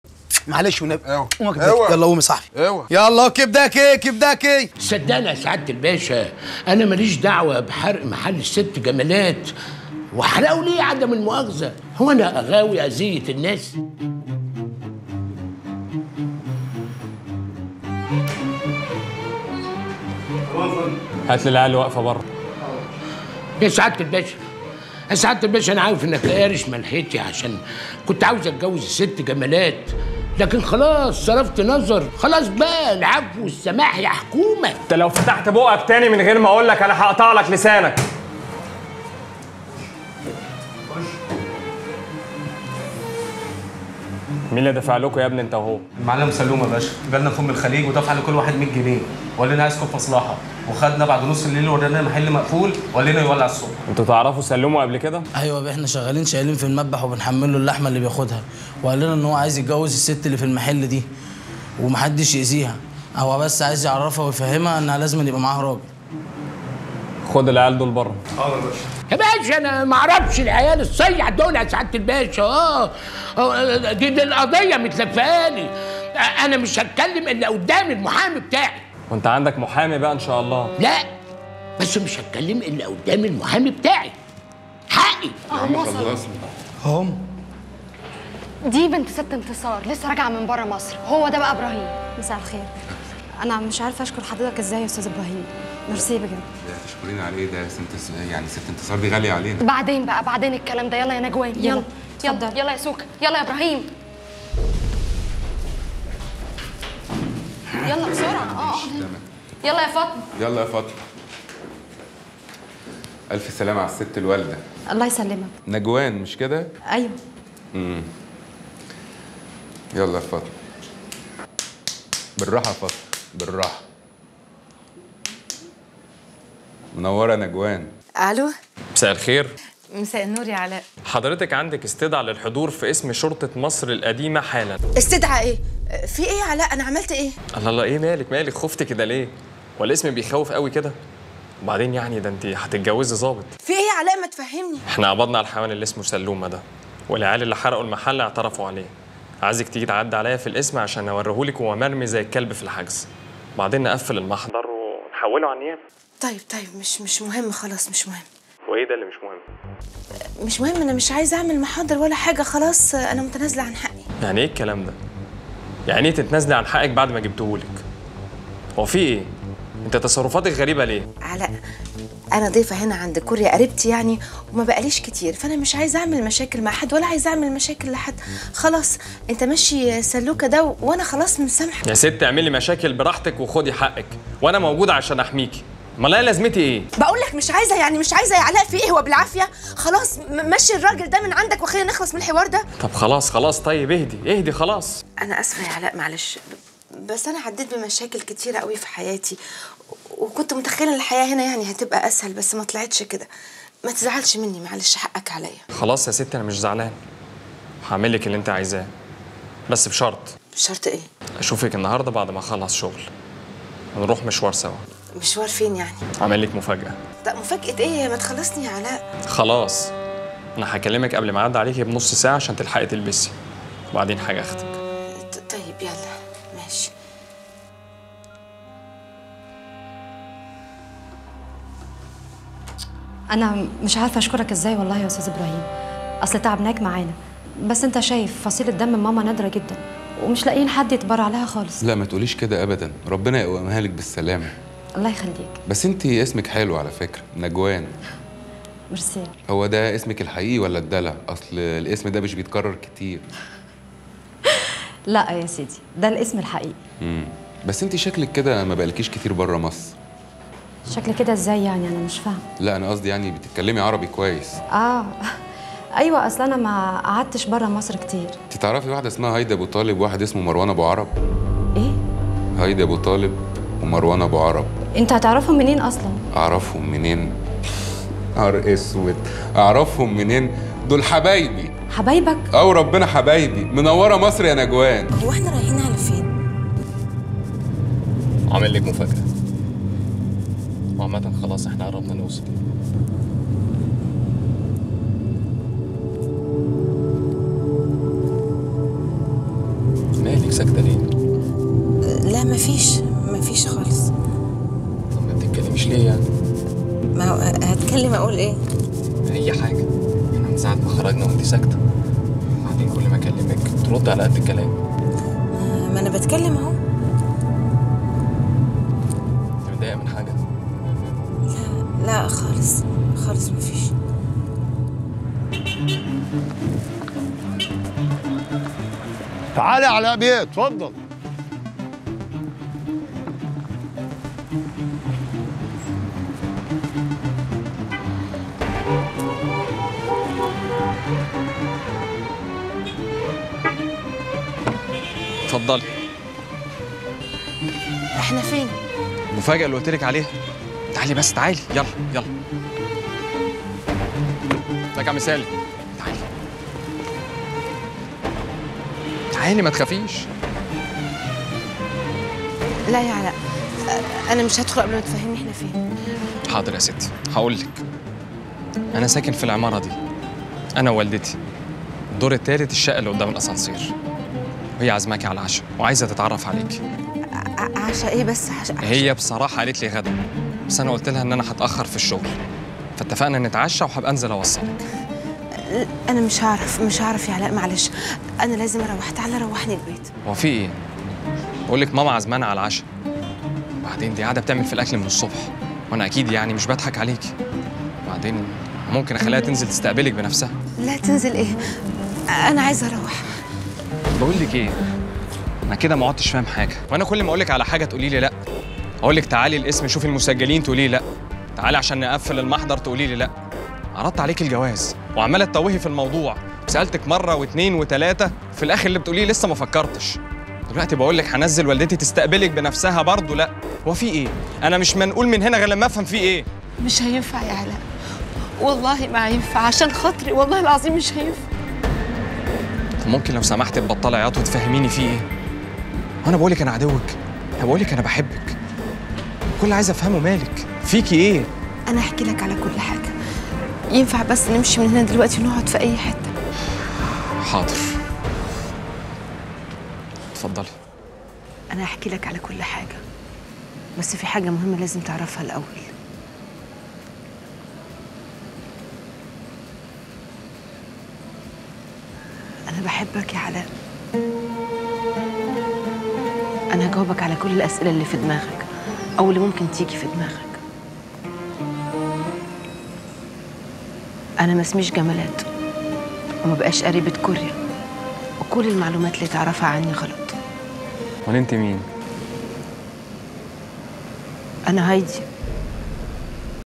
فى اقوى مشهد مؤثر هايدى اعترفت للظابط على الحقيقة
قصة المسلسل

بطولة
مي عز الدين: كوريا عبد العاطي عبد الوهاب
كندة علوش: هايدي أبو طالب سيد السنغوري
ريم البارودي: نوال
محمد إمام:إبراهيم نوح السيد (إبراهيم كبداكي)
نضال الشافعي: مروان أبو عرب
سعد الصغير: سيد (سوكة)
طارق صبري: الضابط علاء عبد الحميد
سناء يوسف: نجوان
محمد سليمان: نادر علم الدين
ياسمين جمال:هاجر
عمر متولي:طاعون صديق إبراهيم
سمير غانم: أبو طالب سيد السنغوري
هند رضا: دينا صديقة هايدي
سارة ناجى:سارة
مصطفى خاطر:طارق محمد سيد السنغوري
فادية عبد الغني:والدة سوكة
كريمة مختار:سندس خالة كوريا
حجاج عبد العظيم:شكري خال كوريا
ضياء الميرغني:سلومة زوج نوال
سامي مغاوري:والد نجوان
حنان سليمان:انتصار والدة نجوان
تامر نبيل:ياسر صديق علاء
نيرة عارف:رجاء علم الدين - أخت نادر
سامح بسيوني:نسر من رجال طاعون